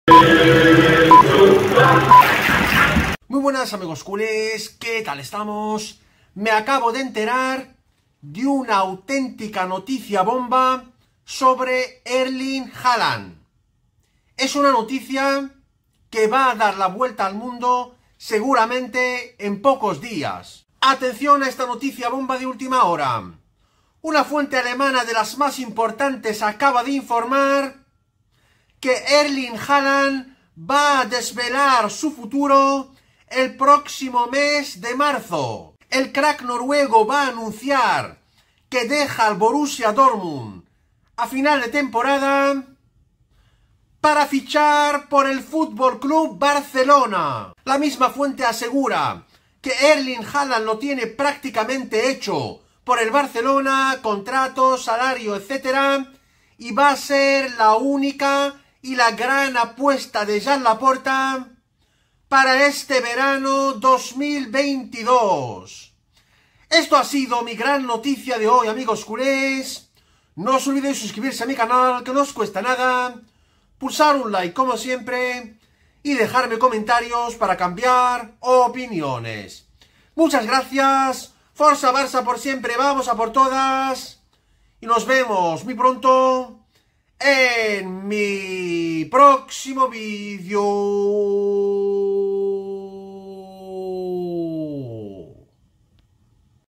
¡¡BOMBA DE ÚLTIMA HORA SOBRE EL FICHAJE DE HAALAND POR EL BARCELONA!!
Bombazo de última hora sobre el fichaje de Haaland por el Barcelona